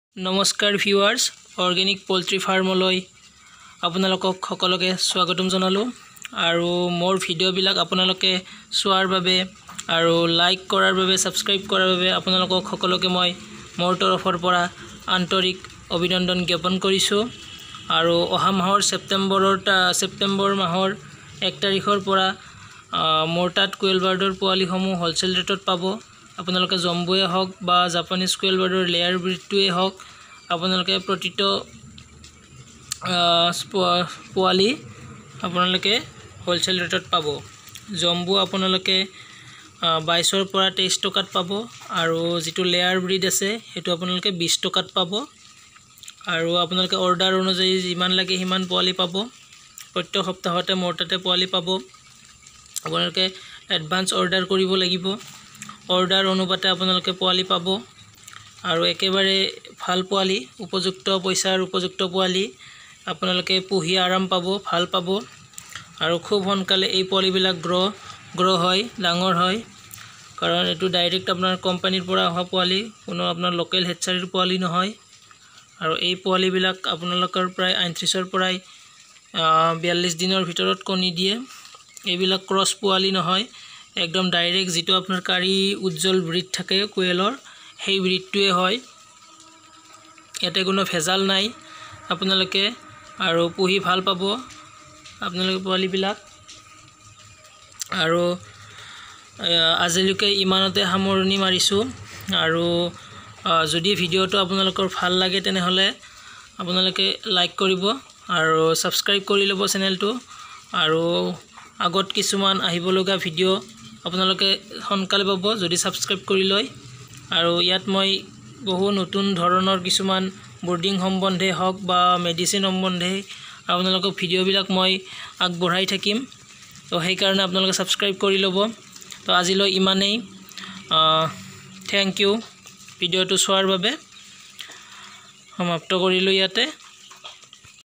Quail birds chiks RS-20 rupees//Nalbari (Assam)//🤳7638000653//Orgenic Poultry Farm//
Orgenic Poultry Farm তৰফৰ পৰা আপোনালোকক সকলোকে আন্তৰিক অভিনন্দন জ্ঞাপন কৰিছোঁ। আৰু আপোনালোকৰ সুবিধাৰ্থে এতিয়া বতাহ চৰাইৰ প্ৰতিটো পোৱালীৰ মূল্য ২০ টকা কৰা হৈছে।  আপোনালোকক যদি বতাহ চৰাইৰ পোৱালী লাগে তেনেহলে আপোনালোকে মোৰ লগত যোগাযোগ কৰিব পাৰে মোৰ contect নম্বৰ টো হল-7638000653